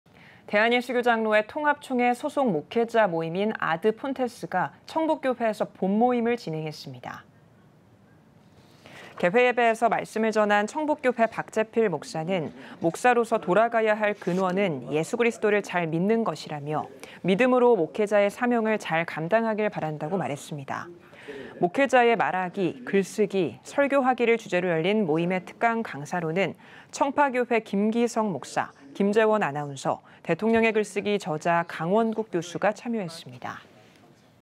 아드폰테스 봄모임 ‘목회자의 말하기, 글쓰기, 설교하기’(충북, 박종혁) l CTS뉴스
대한예수교장로회 (통합)총회 소속목회자 모임인 ‘아드폰테스’가 청북교회에서 봄모임을 진행했습니다.

개회예배에서 말씀을 전한 청북교회 박재필 목사는 “목사로서 돌아가야 할 근원은 예수그리스도를 잘 믿는 것"이라며 "믿음으로 목회자의 사명을 잘 감당하길 바란다”고 전했습니다.

‘목회자의 말하기, 글쓰기, 설교하기’를 주제로 열린 모임의 특강 강사로는 청파교회 김기석 목사, 김재원 아나운서, ‘대통령의 글쓰기’ 저자 강원국 교수가 참여했습니다.